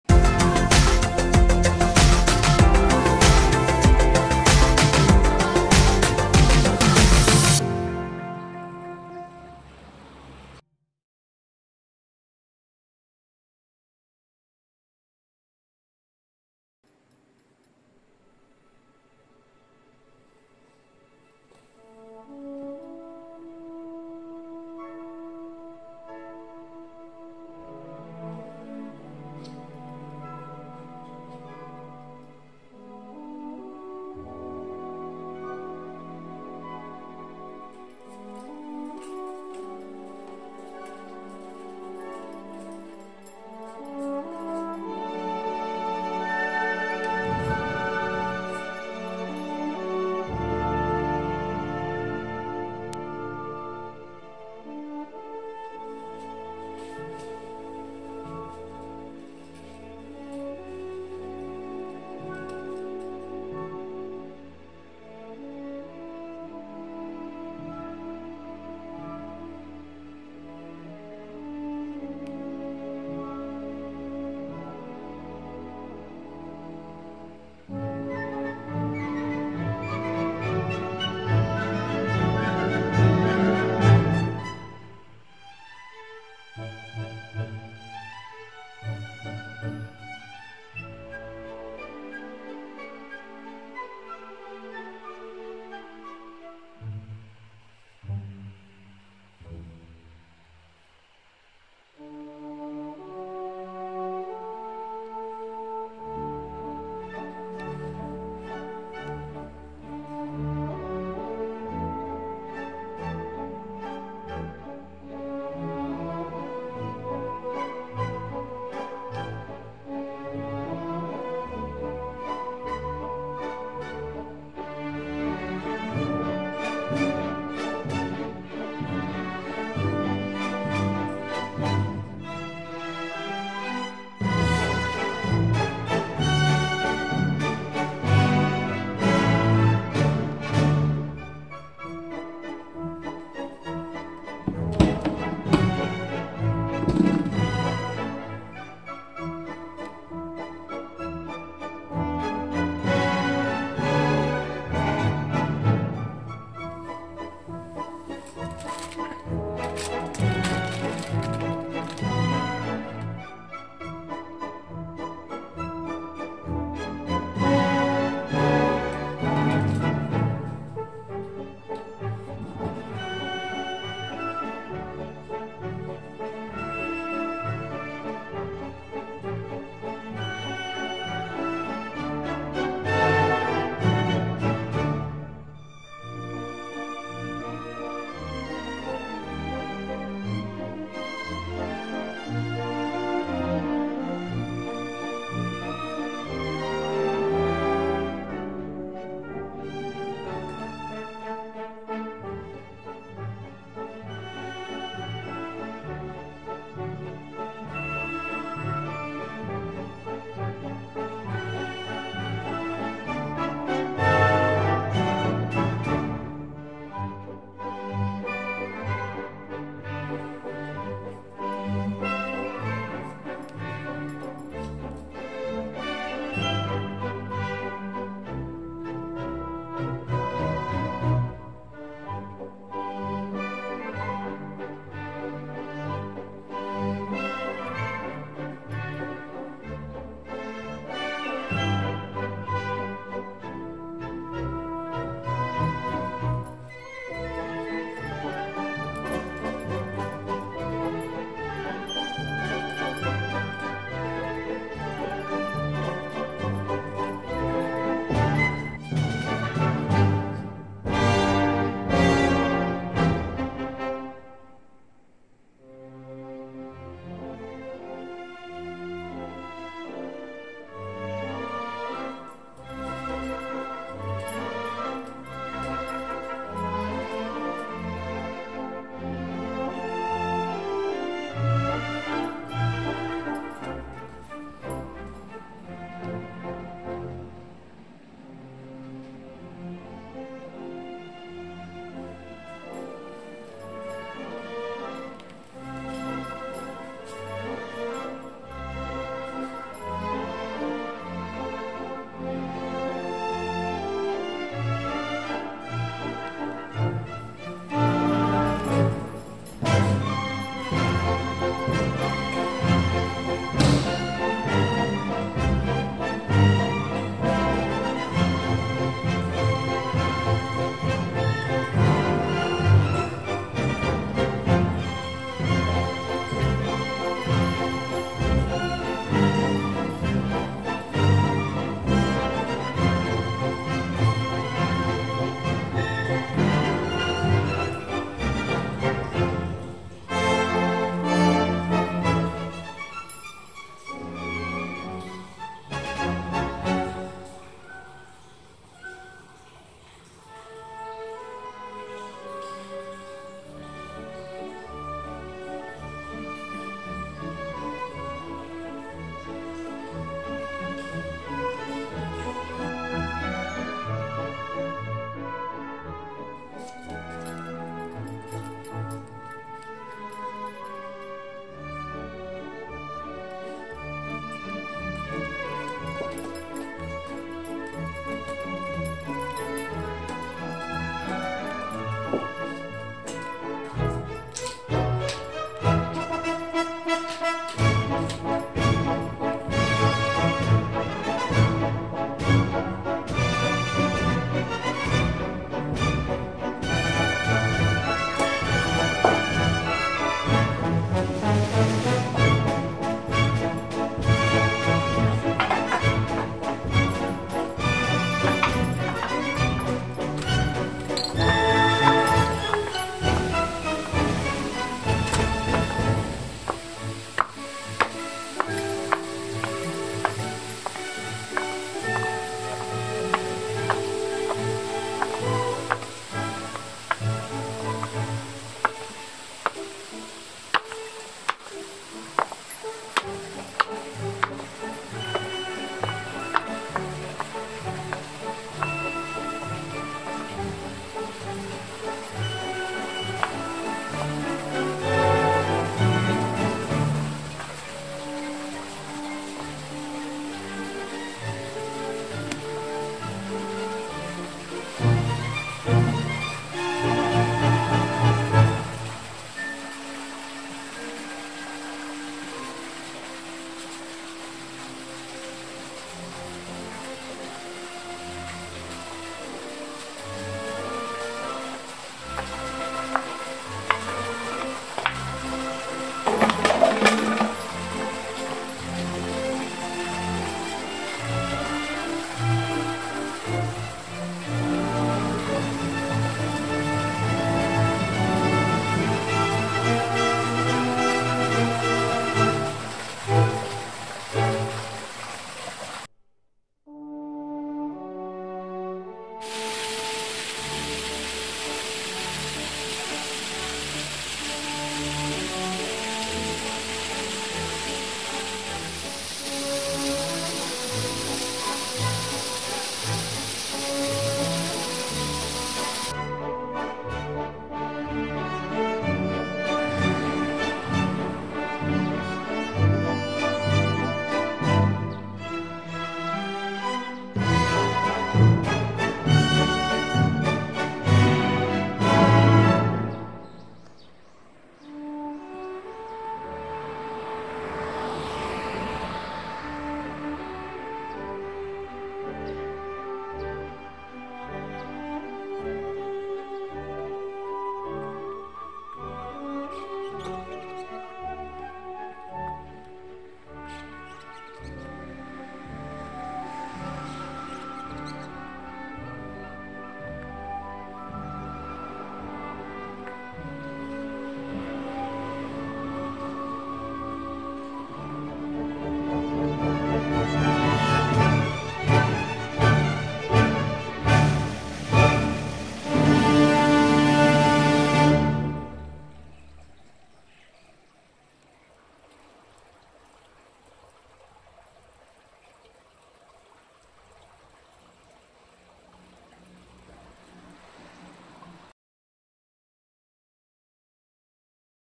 【露天風呂を独り占めする方法】磯部温泉 庭園風露天風呂貸切りの宿 旭館【お一人様ビジネスユース】素泊まり専用になった老舗旅館
【磯部温泉全館制覇トライアル　宿泊施設全7件中5件目】
磯部温泉で駅近の一件。現在は素泊まり専門となっています。
部屋総数は6。古さは否めませんが部屋は綺麗で、防音もしっかりしています。
磯部温泉全般に言えることですが、大浴場への移動は階段利用です。
昭和の雰囲気を味わいたい方にお薦め。
女将さん、頑張っているんで皆さん応援してあげてください。
撮影も編集も投稿者が疲弊してる時期に行ったので、ちょっと雑ですがご了承ください

参考までに群馬サファリパークや富岡製糸場、群馬県立自然史博物館などがある富岡市は
宿泊施設が殆どないので安中市の宿泊施設を利用すると良いかもしれません
また軽井沢へのアクセスもよいので旅の中継地として磯部温泉はおすすめです
磯部温泉の他の旅館や近隣のホテルの動画も別途アップしています